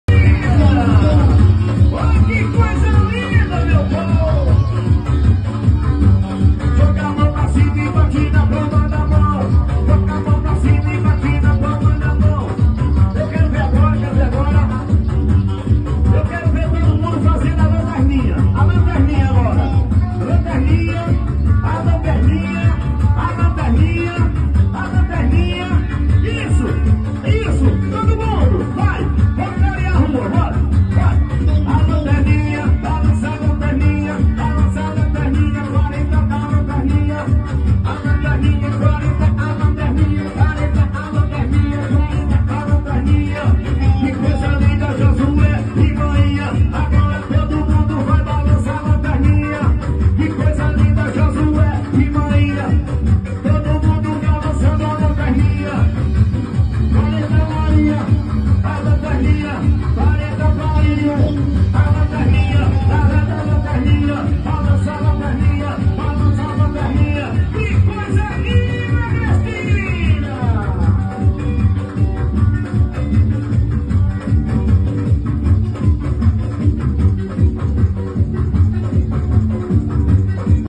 PASSEATA DE JOSUÉ MENDES E CARMEN MIRIAN EM PLENO UMA SEXTA-FEIRA 06/09/2024 EM AGRESTINA-PE
PASSEATA
COMÍCIO
MOTOTOCADA
CARRETA
40 NELES